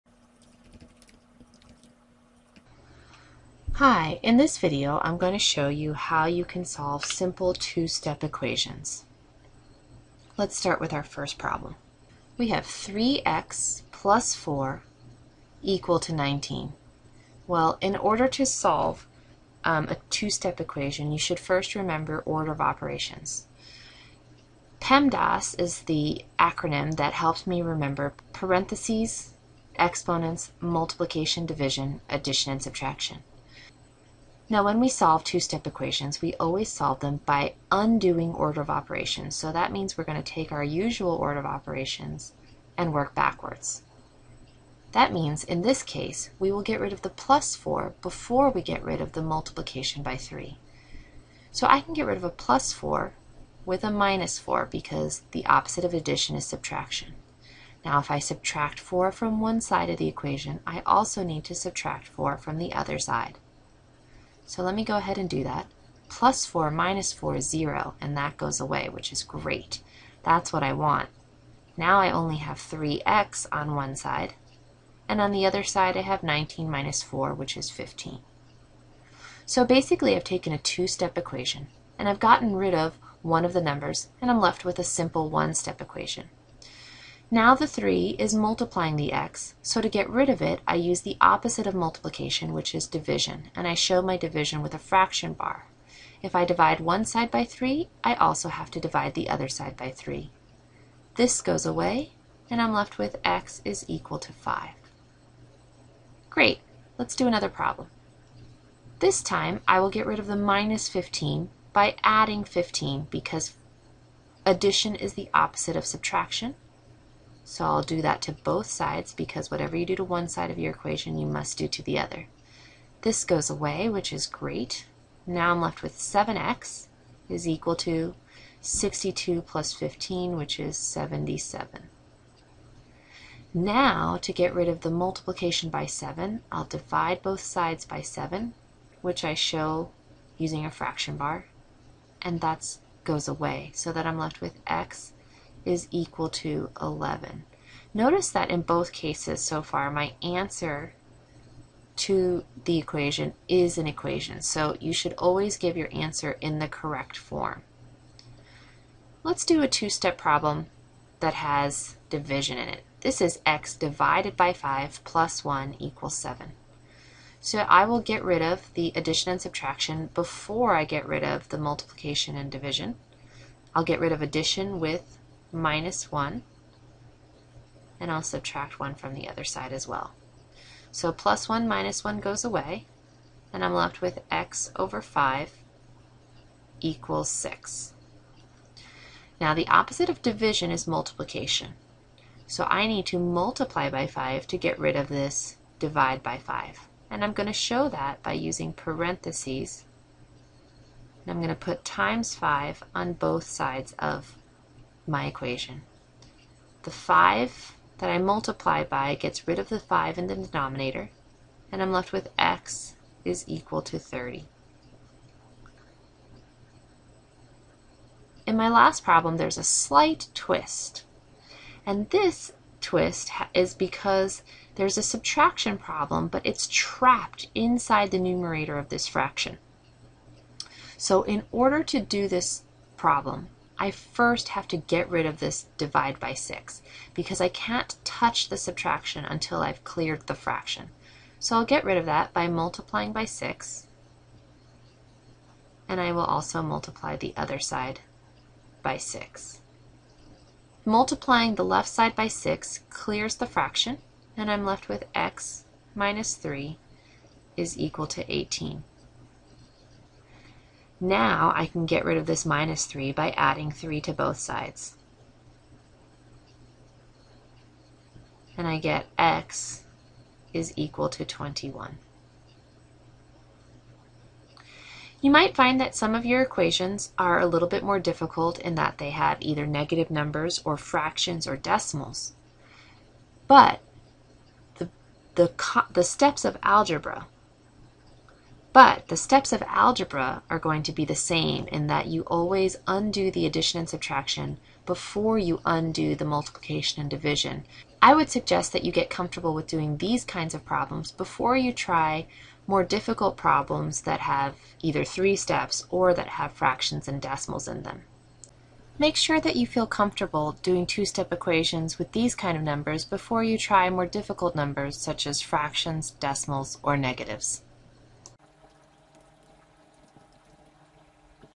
Solving 2-Step Equations - 4 examples.mp4
This video works four simple two-step equations with all positive whole numbers.  The fourth example has subtraction "trapped" inside the numerator, so that you must undo the division first.  Otherwise, these problems are very straightforward two-step equations and would be a logical next step after one-step equations are mastered.